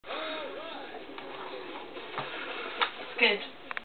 quadruple head spin downhill domination
im sorry to say it wasnt a quadruple headspin but the name came to mind when i made it. i do apolagise but videos will stop for a while because if you look in some of the videos like this one there is a corner of a playstation 2 there and i was trying to fix this problem we had on the t.v and it sort of fell forward and punctured itself on the ps2. sorry for any inconveniance even though hardly anyone watches these...